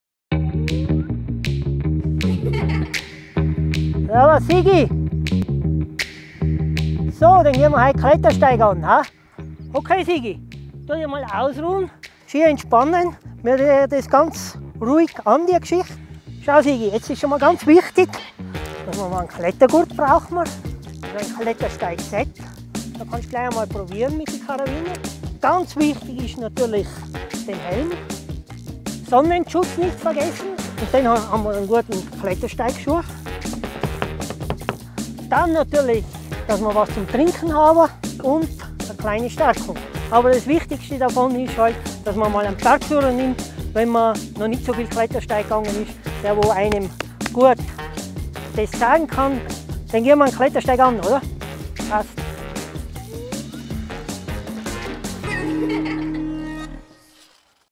Siggi beim Klettern - Galtür
Unser Siggi begleitet Christoph heute bei einem Klettersteig! Auch diesmal erklärt er uns wieder auf was wir alles achten müssen!